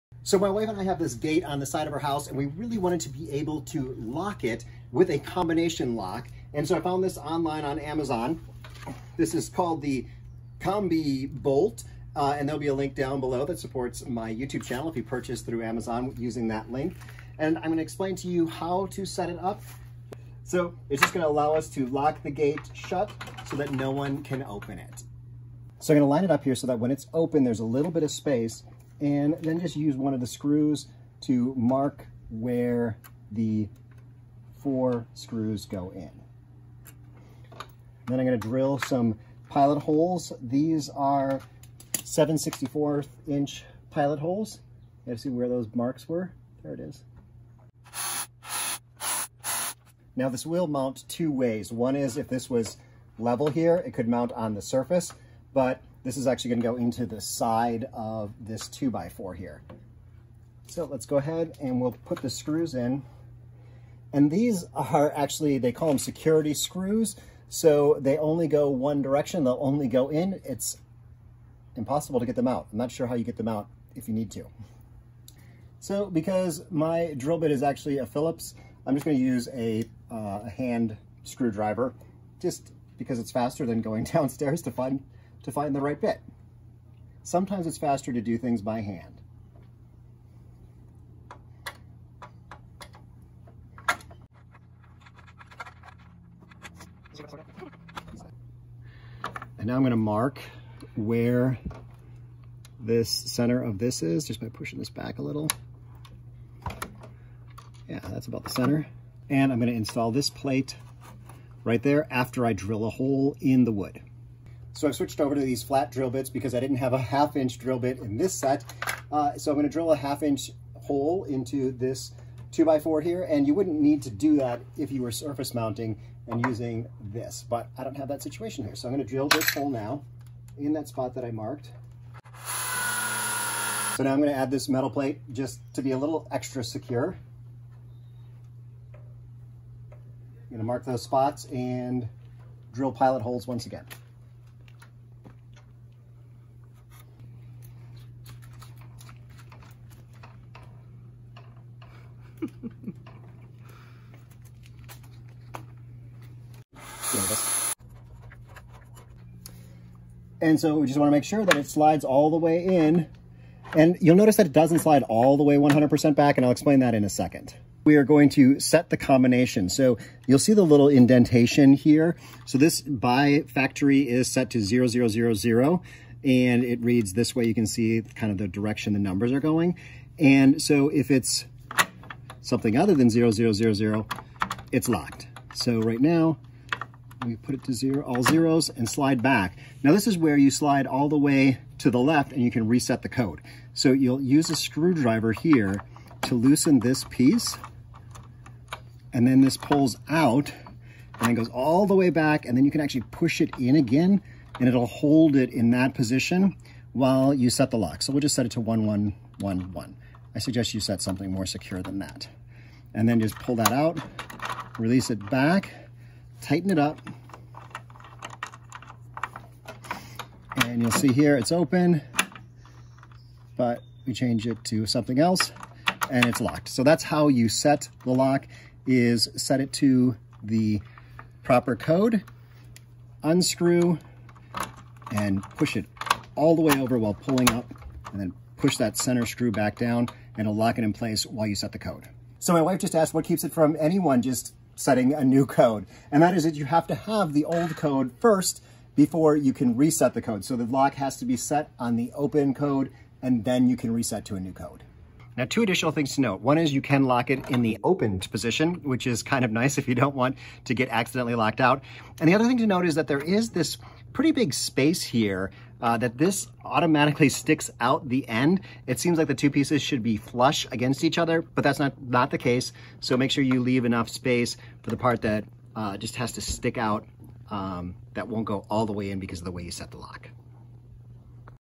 Combination Lock for Gates
I just installed this combination lock on my wooden fence gate.

0:00 Easy to install.
I wanted something other than the standard padlock because it can be hard to see the numbers. These numbers are big and easy to read.

It can install on a flush surface or you can install it into the side of a surface like I do in this video.

0:00 Features
2:47 How to set the combination